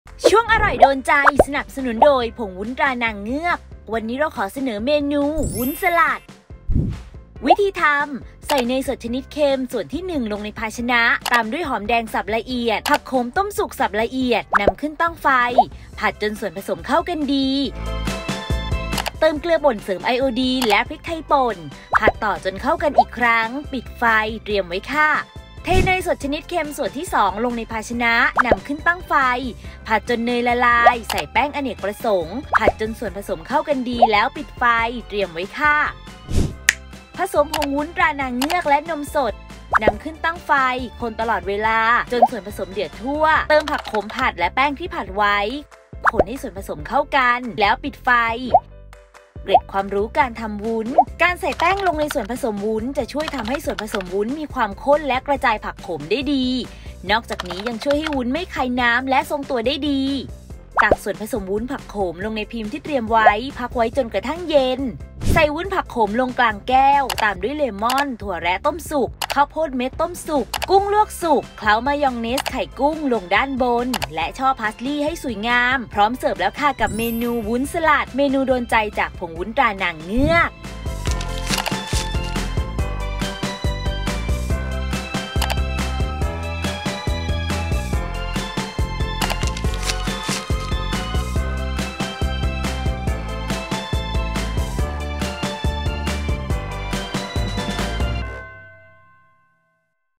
วุ้นสลัด ทำง่ายๆ สไตล์ อ. ยิ่งศักดิ์
วุ้นสลัด (ผลิตภัณฑ์: ผงวุ้น ตรานางเงือก)

เนยสดชนิดเค็ม (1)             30      กรัม
หอมแดงสับละเอียด                         30      กรัม
ผักโขมต้มสุกสับละเอียด                  100    กรัม
เนยสดชนิดเค็ม (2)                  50      กรัม
แป้งอเนกประสงค์                           1       ช้อนโต๊ะ
นมสด     250   มิลลิลิตร
พิมพ์น้ำแข็งกลม 1 พิมพ์
แก้วสำหรับเสิร์ฟ
เลม่อน สำหรับตกแต่ง
ถั่วแระต้มสุก ข้าวโพดเม็ดต้มสุก กุ้งลวกสุกเคล้ามายองเนสและไข่กุ้ง สำหรับรับประทานคู่
ช่อพาร์สลีย์ สำหรับตกแต่ง

1.ใส่เนยสดชนิดเค็ม ส่วนที่1 ลงภาชนะ ตามด้วยหอมแดงสับละเอียด ผักโขมต้มสุกสับละเอียด นำขึ้นตั้งไฟ ผัดจนส่วนผสมเข้ากันดี เติมเกลือป่นเสริมไอโอดีน และพริกไทยป่น ผัดต่อจนเข้ากันอีกครั้ง ปิดไฟ เตรียมไว้
2.เทเนยสดชนิดเค็มส่วนที่2 ลงภาชนะ นำขึ้นตั้งไฟ ผัดจนเนยละลาย ใส่แป้งอเนกประสงค์ ผัดจนส่วนผสมเข้ากันดี ปิดไฟ เตรียมไว้
3.ผสมผงวุ้น ตรานางเงือก และนมสด นำขึ้นตั้งไฟ คนตลอดเวลา จนส่วนผสมเดือดทั่ว เติมผักโขมผัดและแป้งที่ผัดไว้ คนให้ส่วนผสมเข้ากันดี ปิดไฟ(การใส่แป้งลงในส่วนผสมวุ้นจะช่วยทำให้ส่วนผสมวุ้นมีความข้นและกระจายผักโขมได้ดี นอกจากนี้ยังช่วยให้วุ้นไม่คายน้ำ และทรงตัวได้ดี)
4.ตักส่วนผสมวุ้นผักโขมลงในพิมพ์ที่เตรียมไว้ พักไว้จนกระทั้งเย็น เตรียมไว้
5.ใส่วุ้นผักโขมลงกลางแก้ว ตามด้วยเลม่อน ถั่วแระต้มสุก ข้าวโพดเม็ดต้มสุก กุ้งลวกสุกเคล้ามายองเนส ไข่กุ้งลงด้านบน และช่อพาร์สลีย์ ให้สวยงาม พร้อมเสิร์ฟ

▲Line @yingsakfood